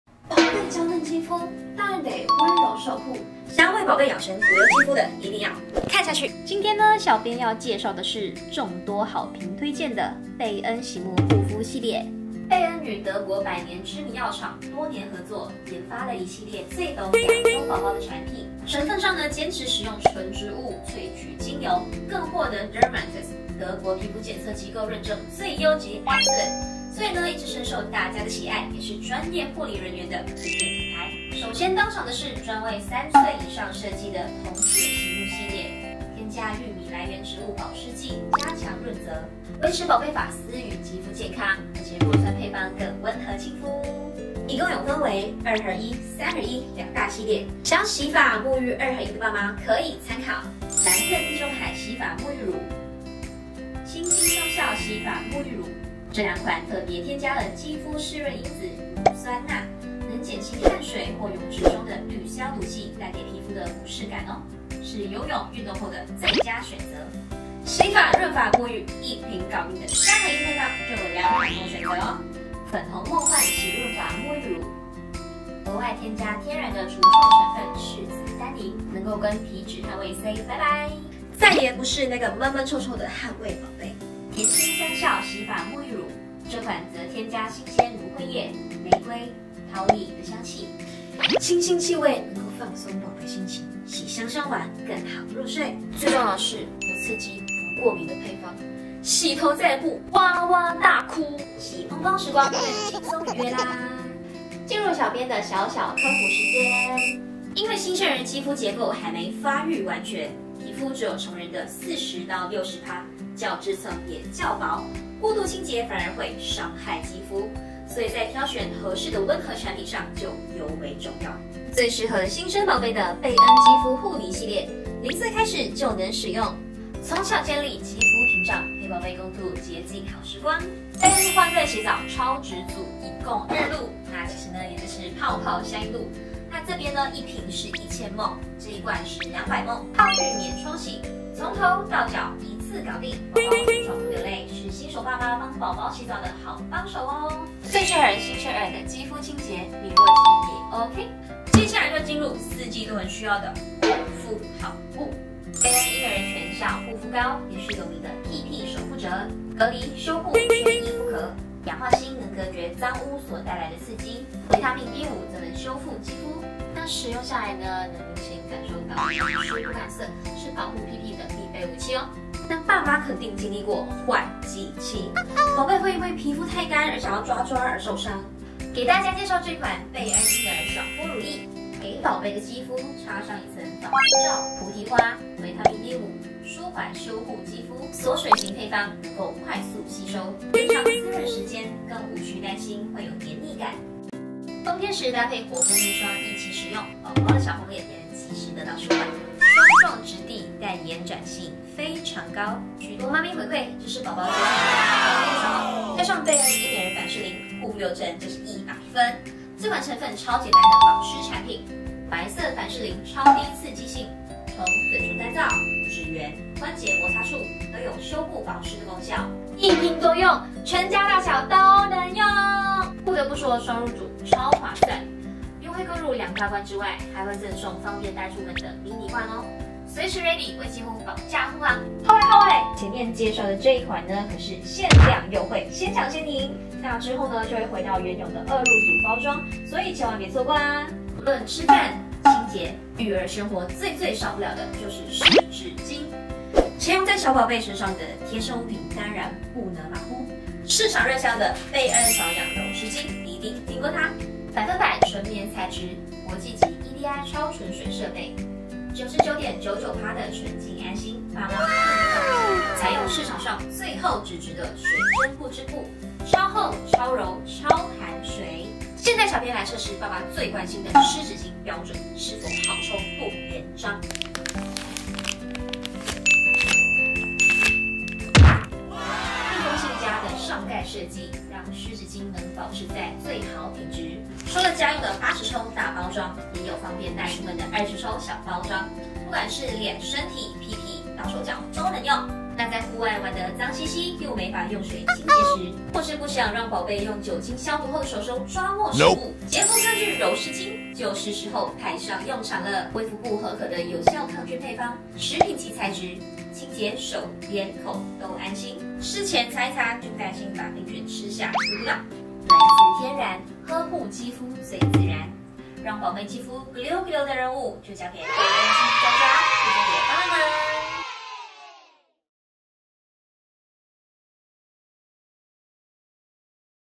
皮膚泛紅？乾燥？交給【貝恩Baan】肌膚溫和護理專家 從洗澡到護膚 給你最全面呵護！
👶容易泛紅、乾燥？屁屁反復起疹？
還在為寶貝肌膚問題困擾的爸媽看過來！

與德國知名百年藥廠DREITURM多年合作
研發了一系列最懂亞洲寶寶的產品

✔ Dermatest德國皮膚檢測機構認證最優級
✔ 專業護理人員的推薦品牌

新品搶先報！3歲以上幼童適用的 童趣洗沐系列
有二合一、三合一兩大配方，四款不同清新香味
添加乳酸鈉，運動寶貝不被汗水不適感打擾
也跟皮脂汗味say bye bye~~

💡 進入0歲小嫩嬰就能使用的全面介紹 💡

新生兒必備的泡泡香浴露
泡浴免沖洗，新手爸媽的洗澡好幫手
嬰兒全效護膚膏 有名的屁屁守護者
隔絕髒污、修復肌膚，有效改善紅屁屁！

換季時期皮膚太乾而抓傷可不行！
給大家介紹 嬰兒爽膚乳液
鎖水型配快速吸收、更無需擔心黏膩感
冬天時搭配眾多媽咪推薦的 活膚面霜
小紅臉能及時得到舒緩，霜狀質地但延展性很高！

嬰兒凡士林 就是全家人必備
從嘴唇乾燥、指緣、關節摩擦處
都有效修復保濕的功效 💯

嫩嬰日常當然少不了 貝恩保養柔濕巾
✔市面上最厚紙質 ✔不連抽 ✔國際級超純水
食品級材質的 潔膚抗菌柔濕巾
衛福部核可有效抗菌配方，清潔手臉口都安心